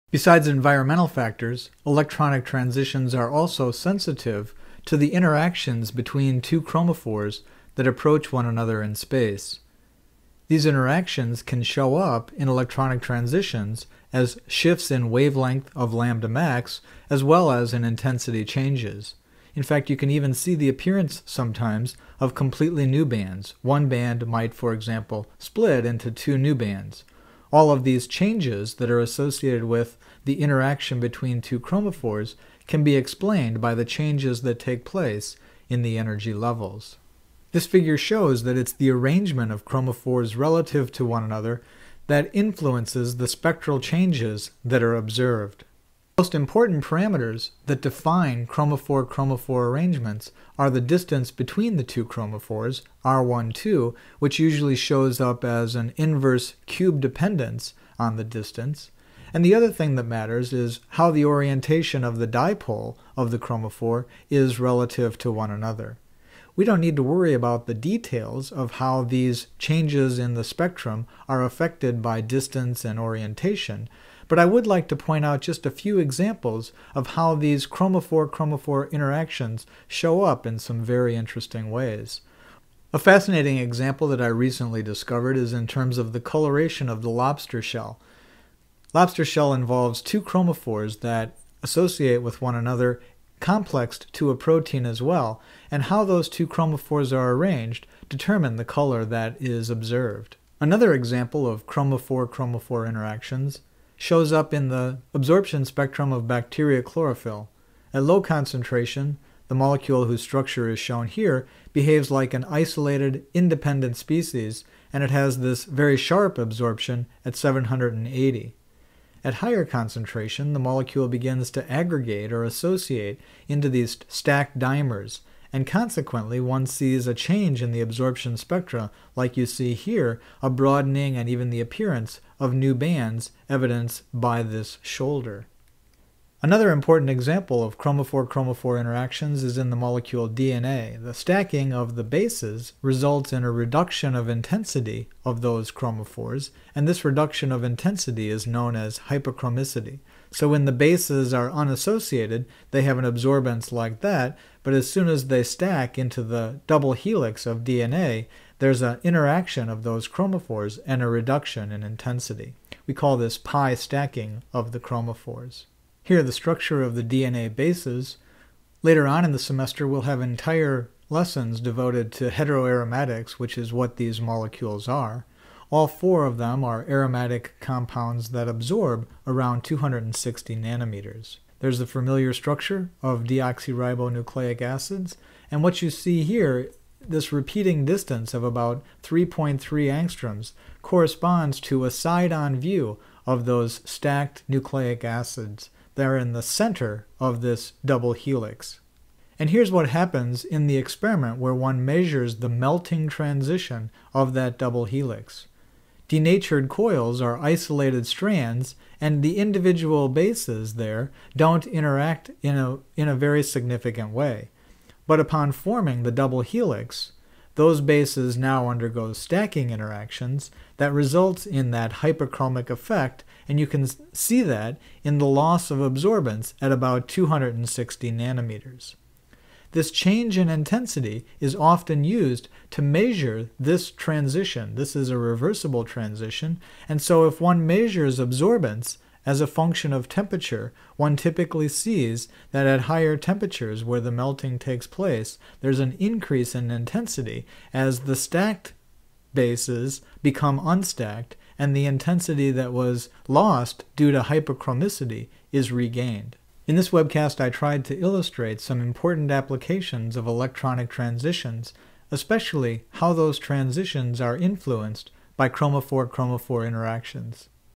Chromophore-Chromophore Interactions (3.5)
This webcast describes a few applications of electronic absorption spectroscopy to study the interaction of chromophores.